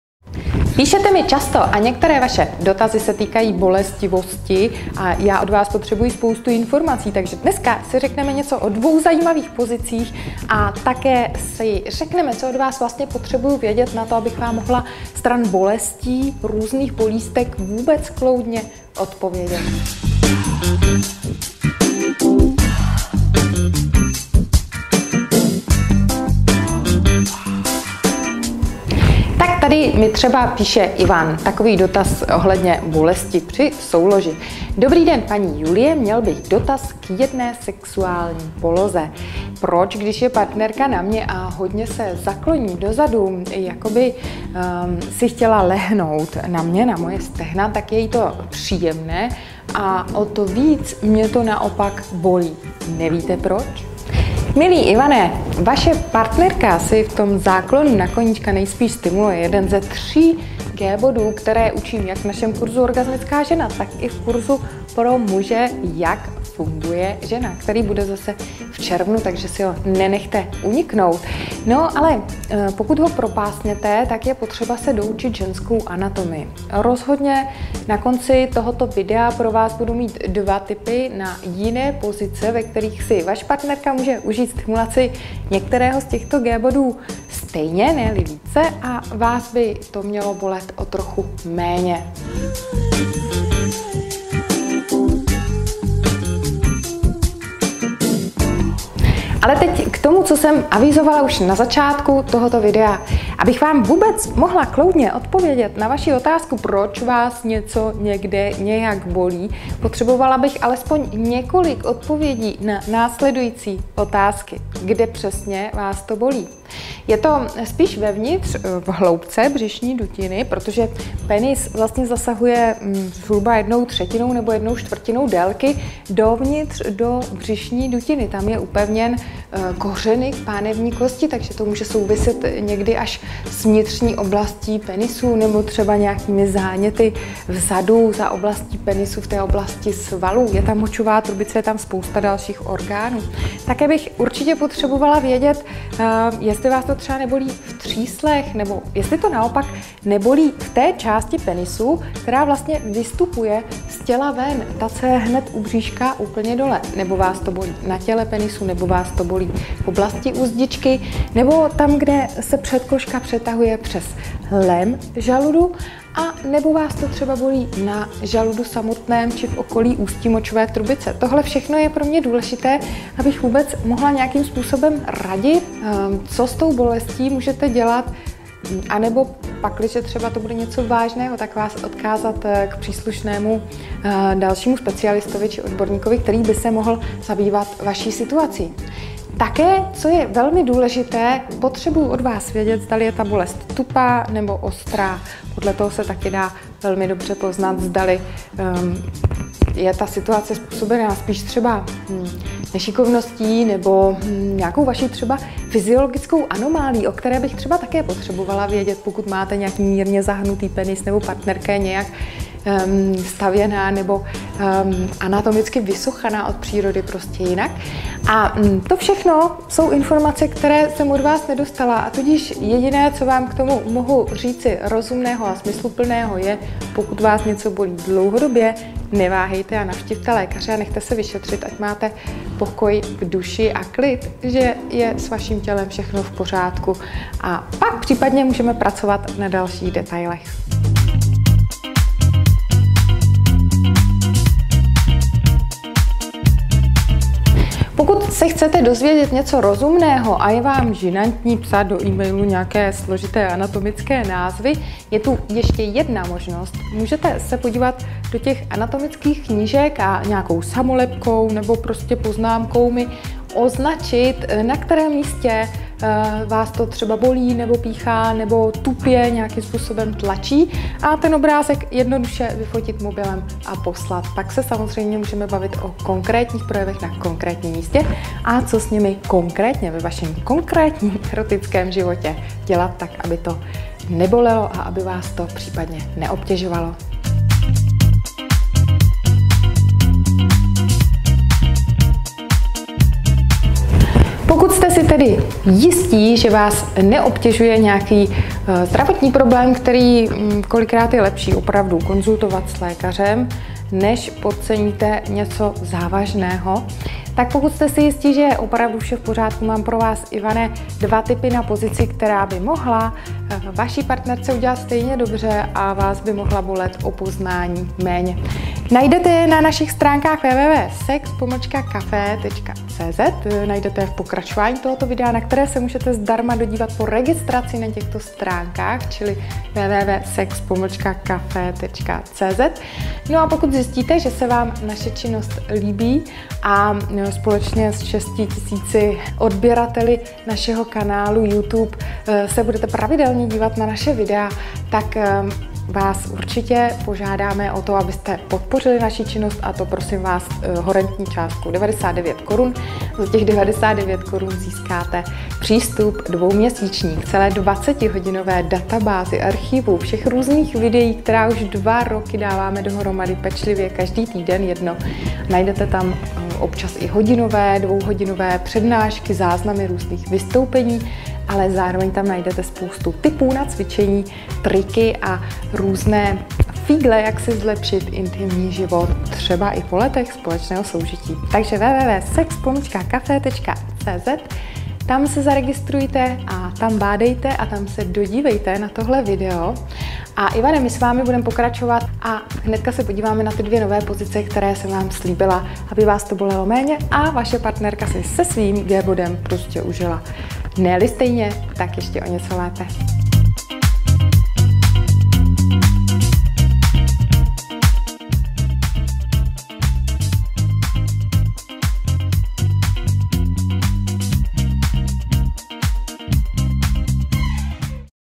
Když penis bolí v pozici na koníčka | SExCAFÉ
Ivan se ptá, proč, když je partnerka v pozici na koníčka a zaklání se, jí to extrémně vzrušuje, zatímco jeho to naopak bolí. Odpověď najdete v SExCAFÉ.